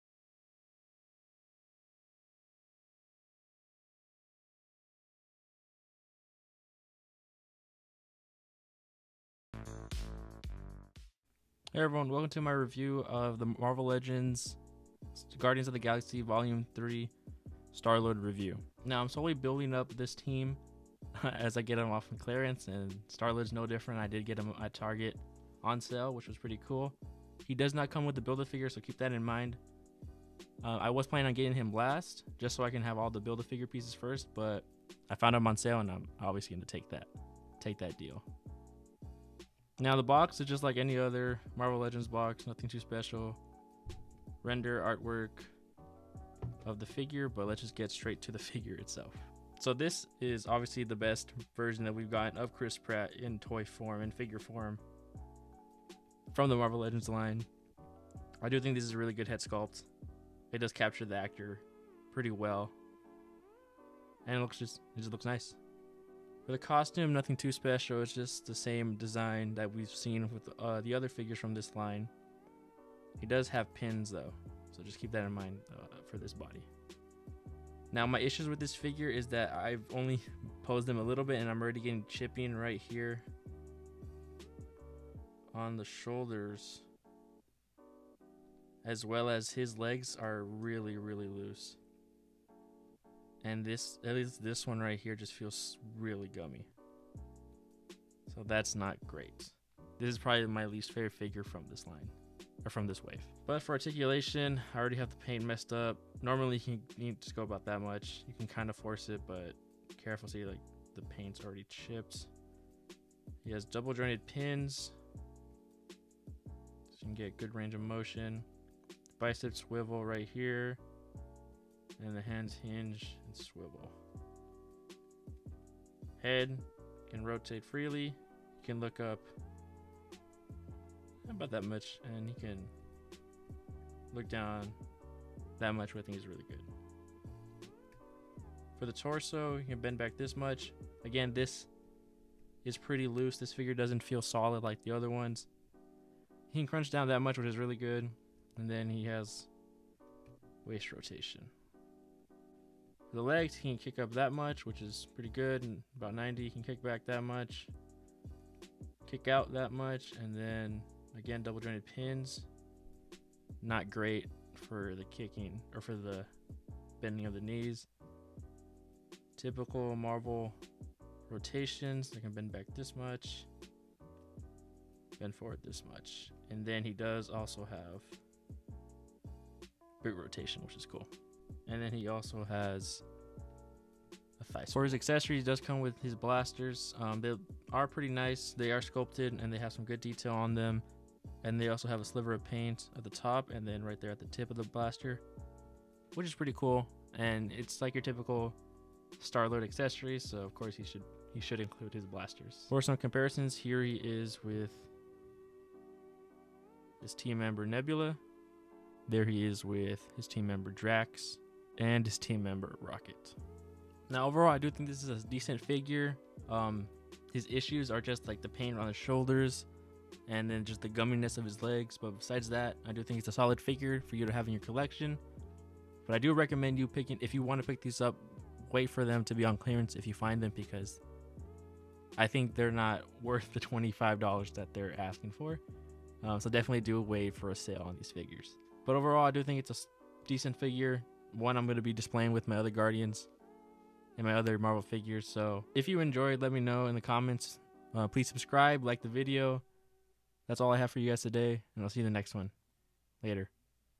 Marvel Legends | Guardians of the Galaxy Vol 3 | Star-Lord | Build-a-Figure | Review
Today I continue my reviews on the Marvel Legends Guardians of the Galaxy Build a Figure wave Star-Lord.